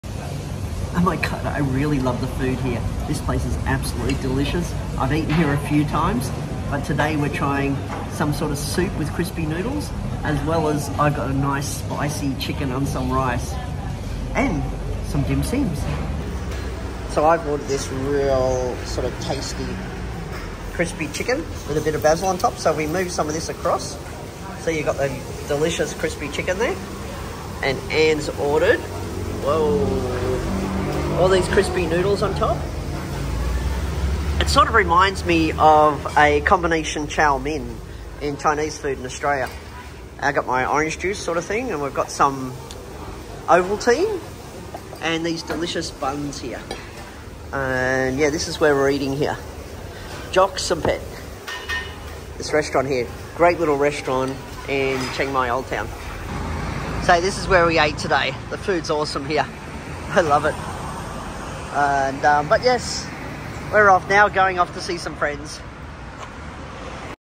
Delicious Thai Food at Jok Somphet Chiang Mai Thailand | Chiang Mai Food Vlog
Delicious Thai Food at Jok Somphet Chiang Mai Thailand | Chiang Mai Food Vlog | This is a must try if you are in Chiang Mai as the food is cheap and delicious.